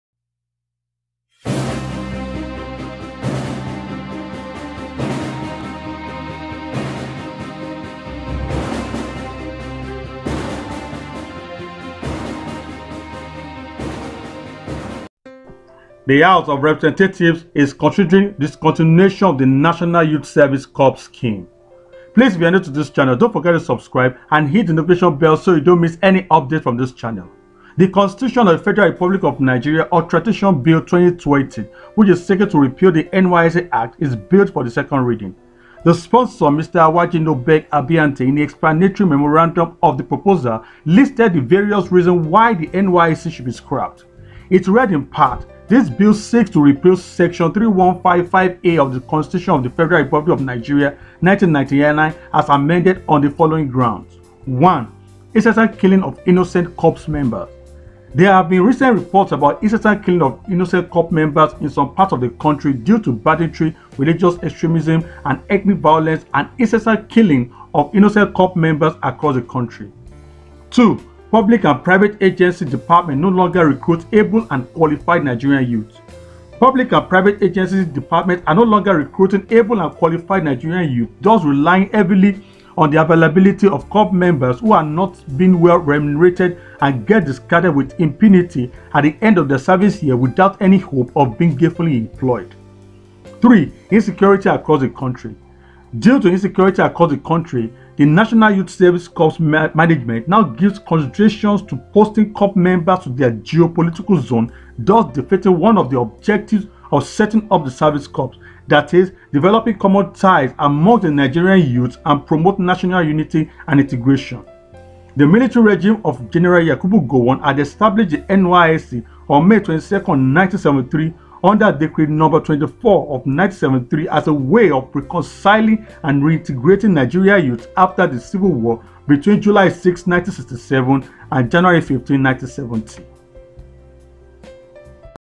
Reps consider scrapping NYSC For these 3 Reasons as bill reaches second reading
THE House of Representatives is considering discontinuation of the National Youth Service Corps scheme.